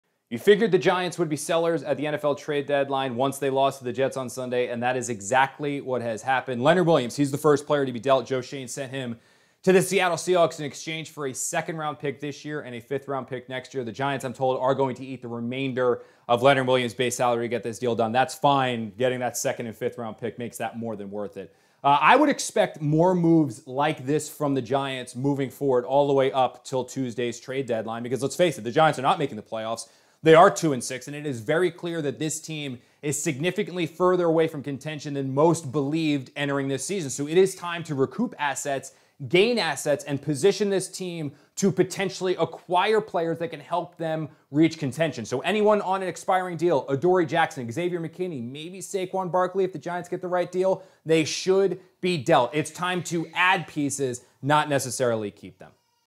Giants trade defensive end Leonard Williams to Seahawks for draft picks | SNY
As the NFL trade deadline approaches, SNY's NFL Insider Connor Hughes reacts to the Giants decision to trade defensive end Leonard Williams to the Seahawks for multiple draft picks.